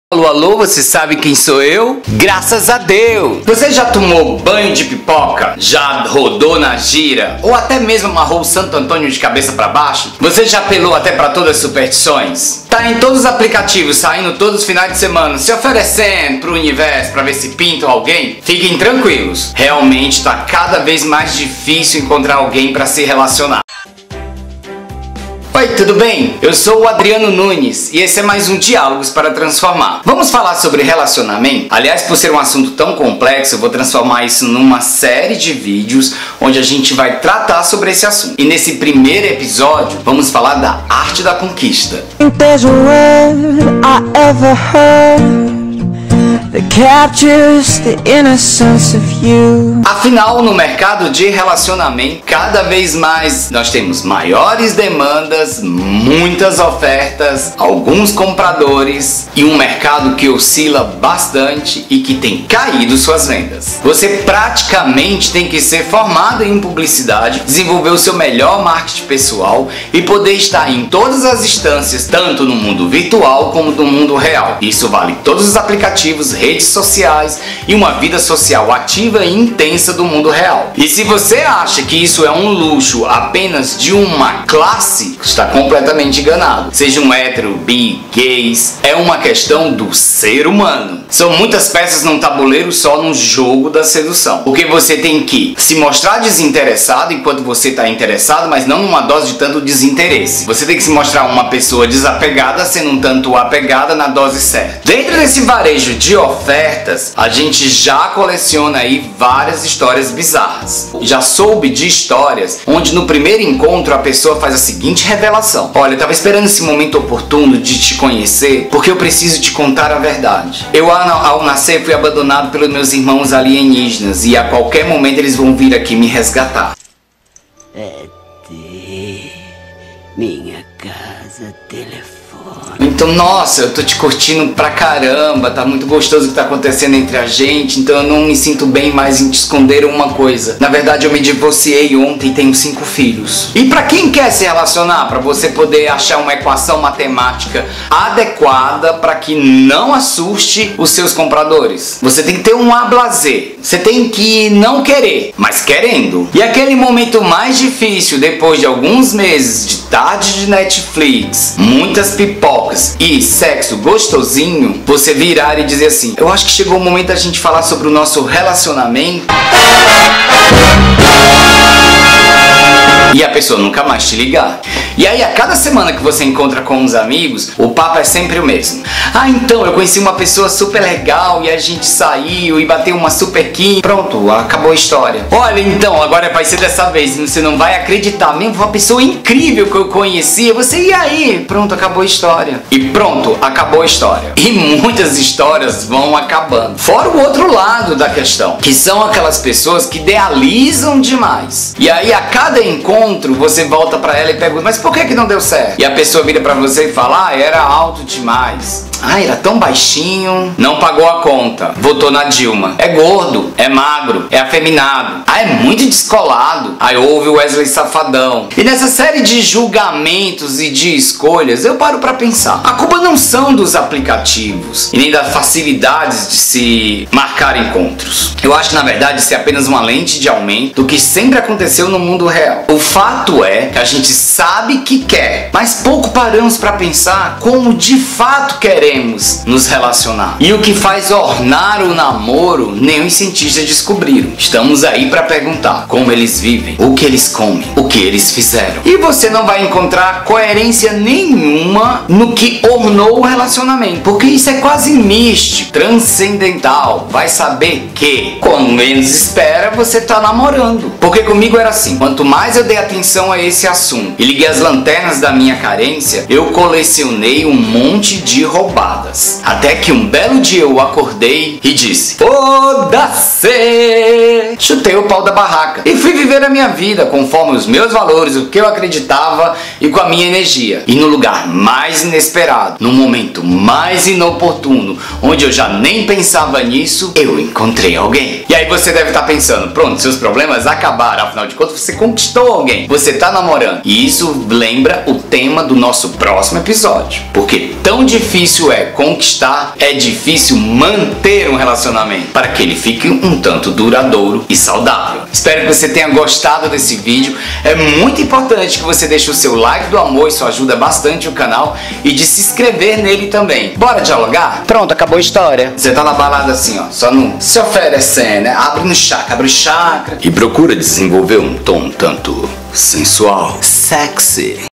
SÉRIE RELACIONAMENTO: COMO CONQUISTAR UM NAMORADO / ADRIANO NUNES - DIALOGAY
Tá cada vez mais difícil conquistar um namorado? Neste episódio vou falar que um bom mesmo é relaxar e deixar as superstições de lado!  Boas dicas para desenrolar um namoro... se ofereçaaa...